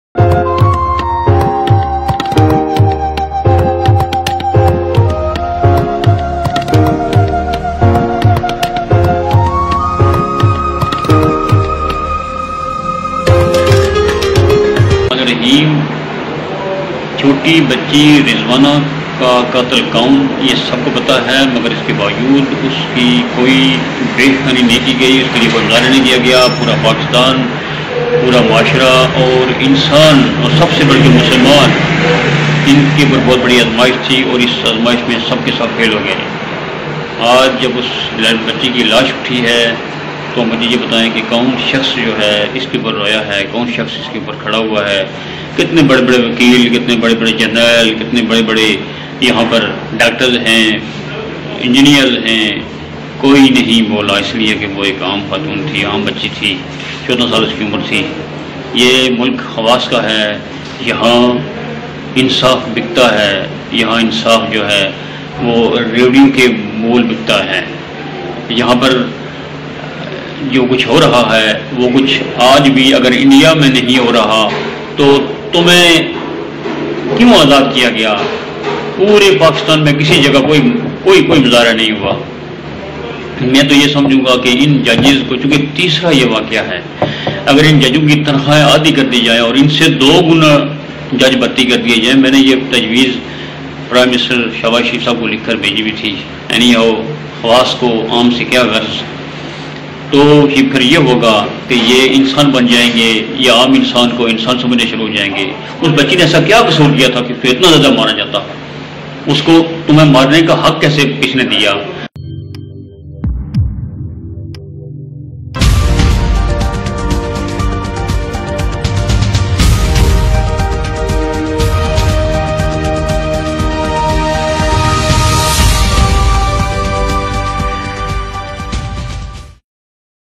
معصوم رضوانہ کواںصاف کون دے گا۔ جج کی بیوی کو پھانسی ہونی چاہیئے
Who will give justice to a working child in a Judge House